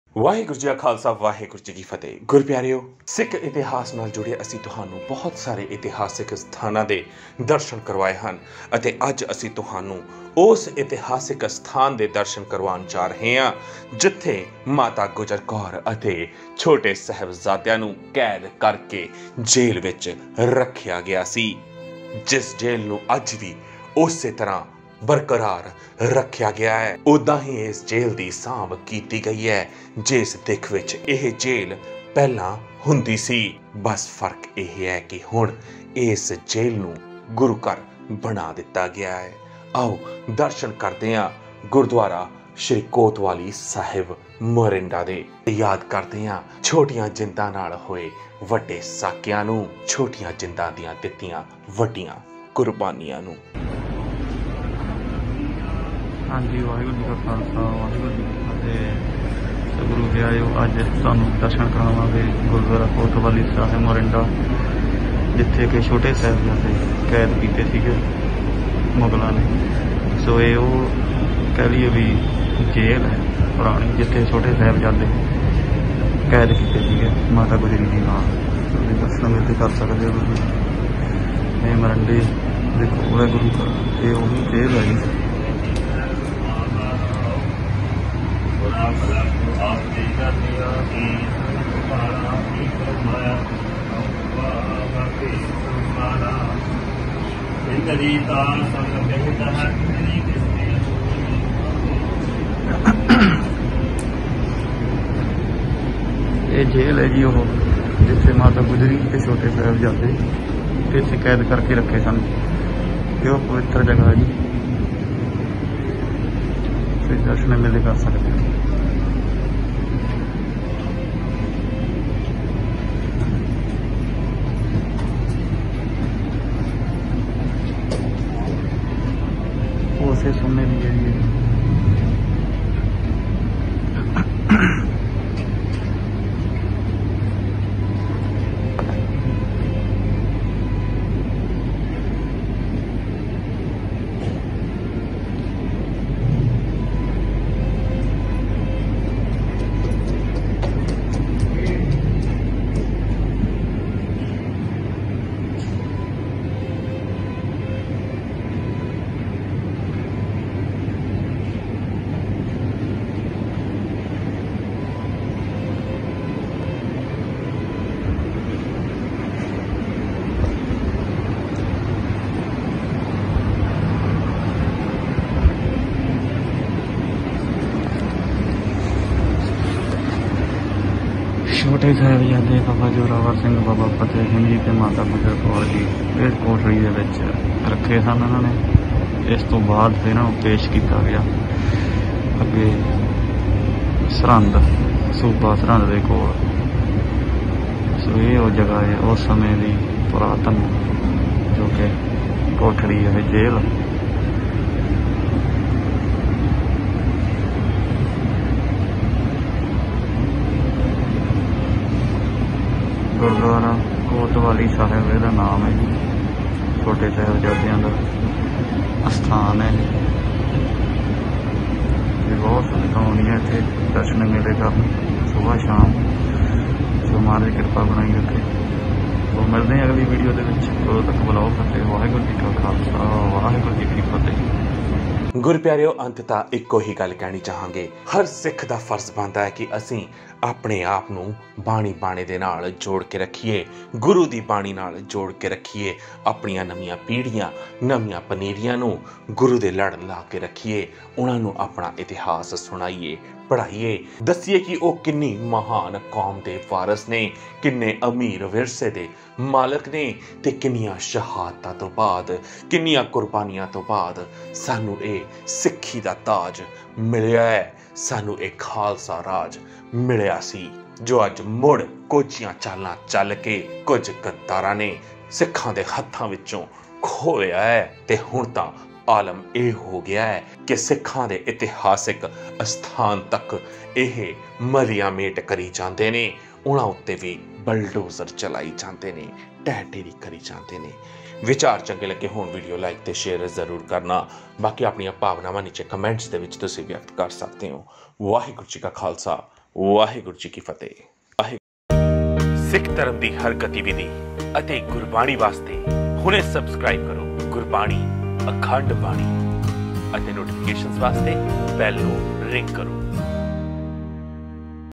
ਦਰਸ਼ਨ ਕਰੋ ਉਸ ਜੇਲ ਦੇ ਜਿੱਥੇ ਮਾਤਾ ਗੁਜਰ ਕੌਰ ਅਤੇ ਛੋਟੇ ਸਾਹਿਬਜ਼ਾਦਿਆਂ ਤੇ ਤਸ਼ੱਦਤ ਢਾਹੀ ਗਈਸੀ | Gurbani Akhand Bani
Must Watch and Visit The Historical Place That Jail Where the Crime was Committed against Mata Gujar Kaur ,Grandmother's Grandson Sahibzade .